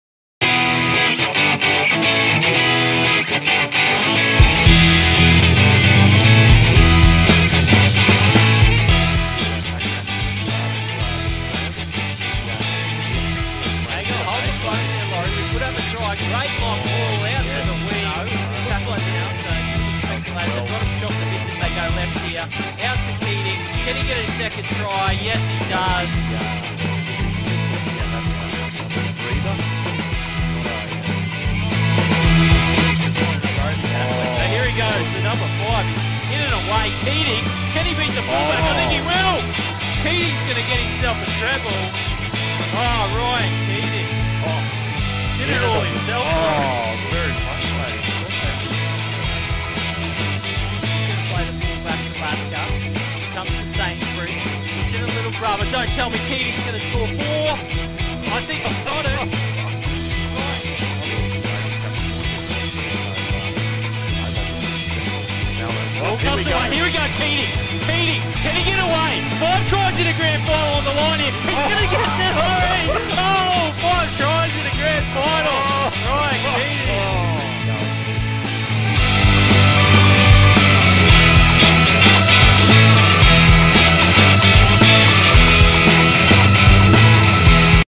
Ryan Keating (SBC) 5 tries in a GF
SBC Winger Ryan Keating scores five tries in the 2021 RDSSRL Open B grand-finals